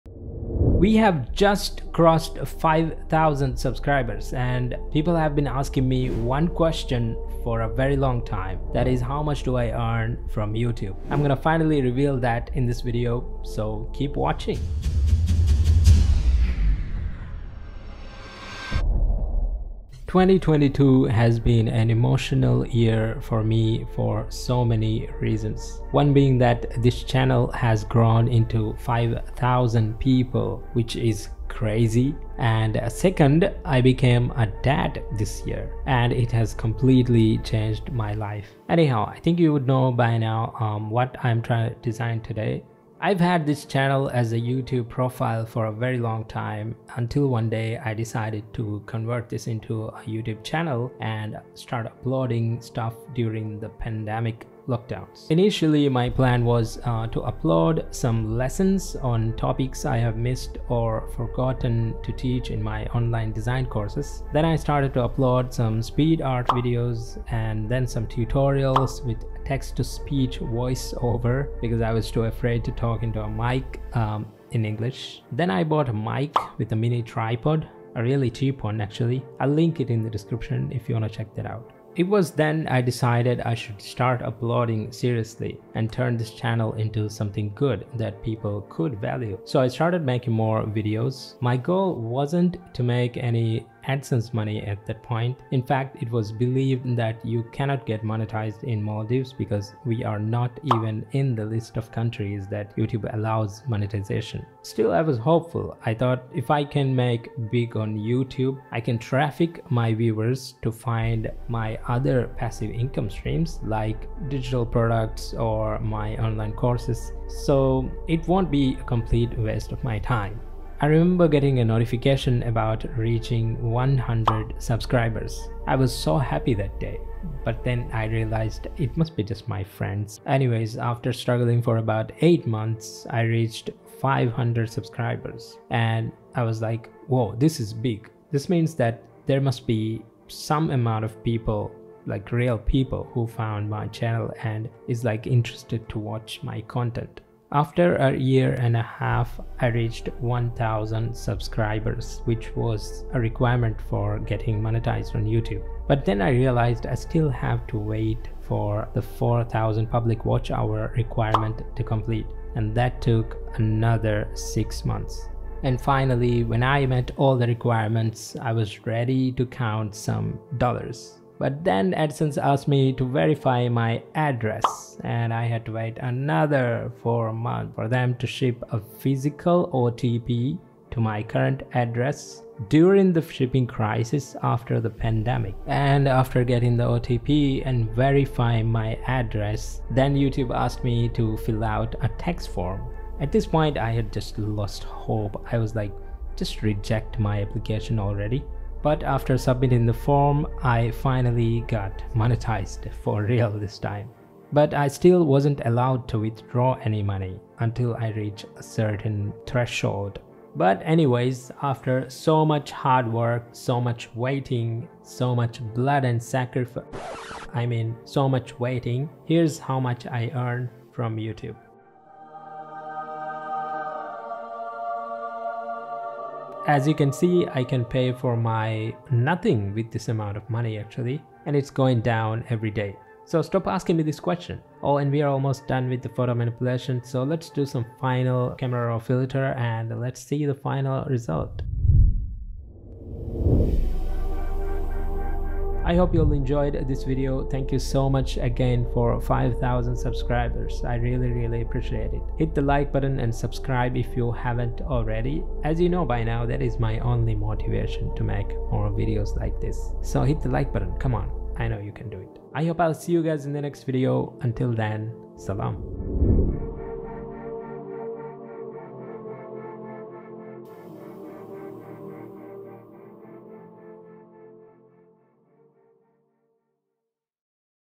How much YouTube pays me for 5k subscribers
In this video, I'm revealing how much I earn from YouTube AdSense revenue while also designing an apocalyptic photomanipulation.

Materials used: @envato elements, @unsplash_

Outro: @sieddofficial